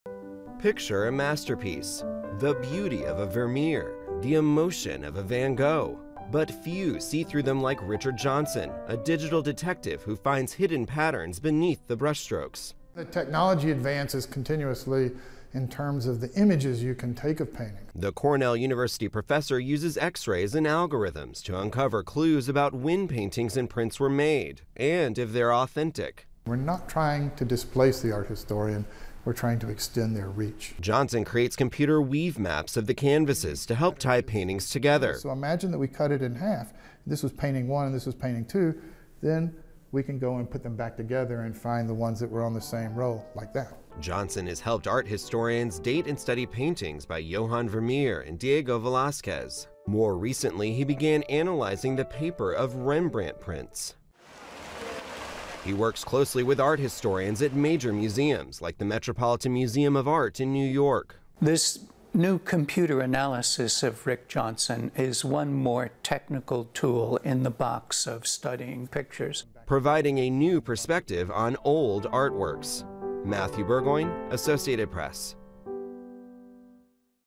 Professor Unlocks the Mystery of Paintings
Richard Johnson, a computer and engineering professor at Cornell University, is using technology to uncover mysteries about the age and authenticity of historic paintings by artists like Johannes Vermeer and Vincent Van Gogh. (Nov. 19)